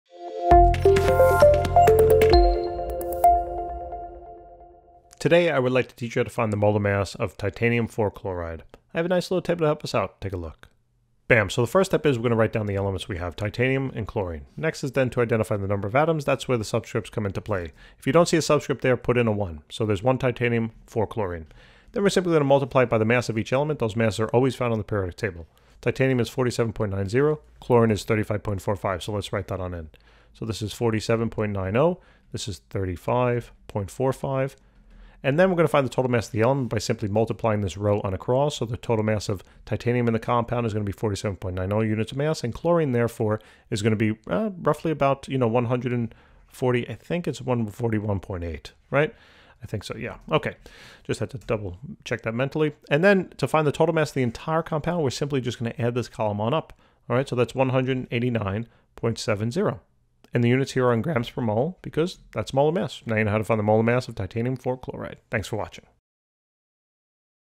How to find the molar mass of TiCl4 (Titanium (IV) Chloride)
Calculate the molar mass of the following:
TiCl4 (Titanium (IV) Chloride)